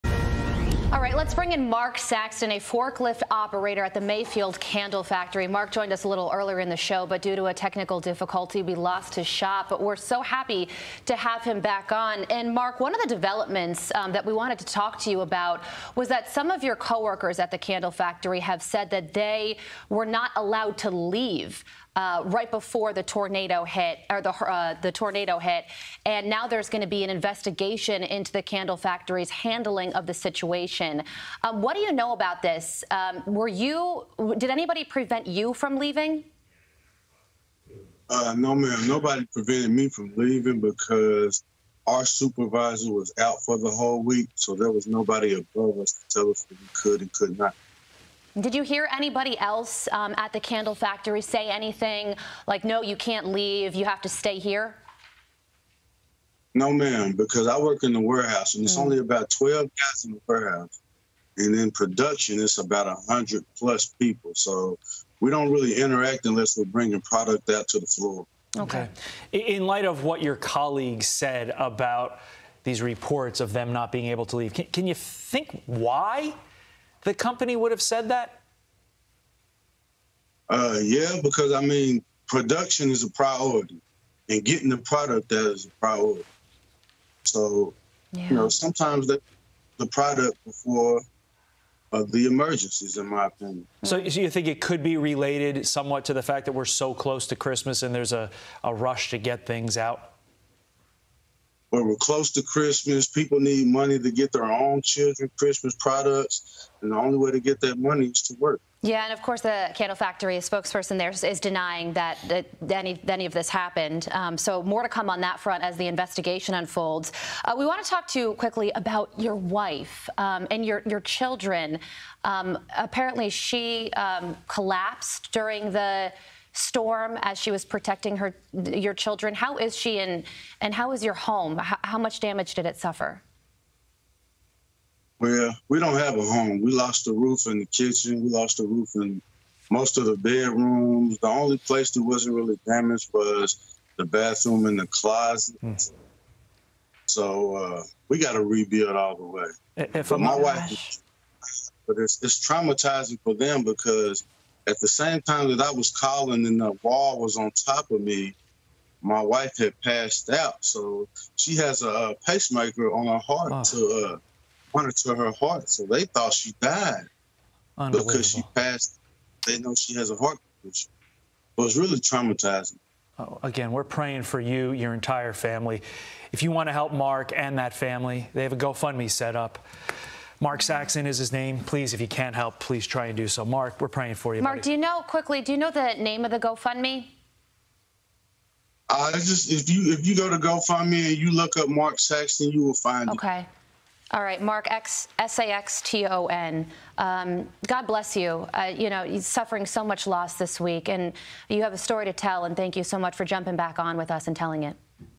Tornado survivor recalls ‘traumatizing’ experience: ‘We don’t have a home’
Forklift operator Mark Saxton discusses the impact of the tornado on his family and his place of work.